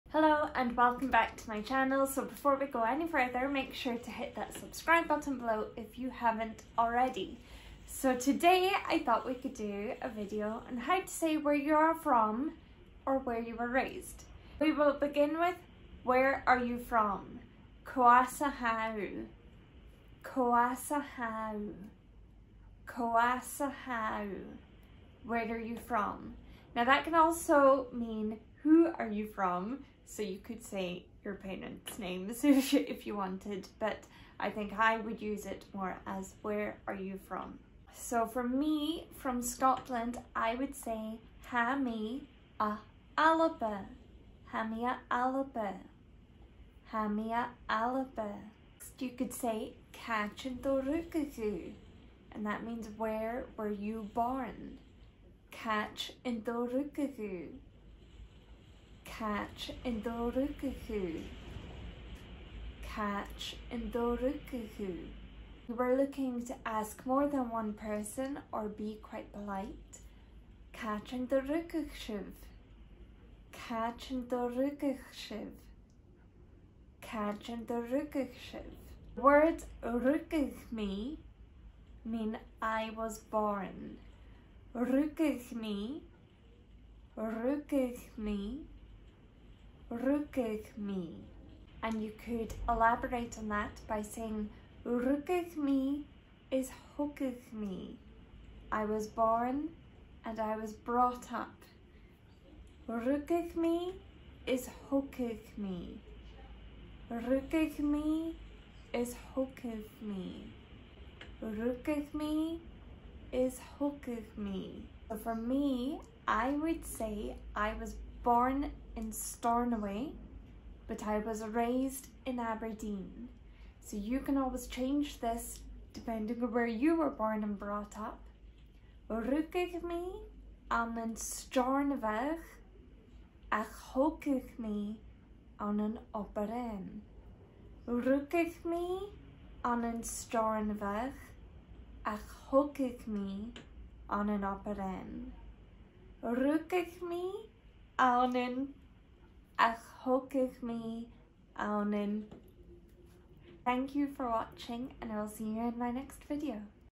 How To Ask "Where Are You From?" In Scottish Gaelic (With Phonetics) | Learn Scottish Gaelic
Cò às a tha thu? - Who are you from/Where are you from?
Tha mi a.. (Alba) - I am from...
Càit an do rugadh tu?- Where were you born?
Càit an do rugadh sibh? - Plural/polite - Where were you born?
Rugadh mi.. - I was born..
Rugadh is thogadh mi ann.. - I was born and brought up..
Rugadh mi ann an… ach thogadh mi ann an.. - I was born in.. but brought up in.

Lauren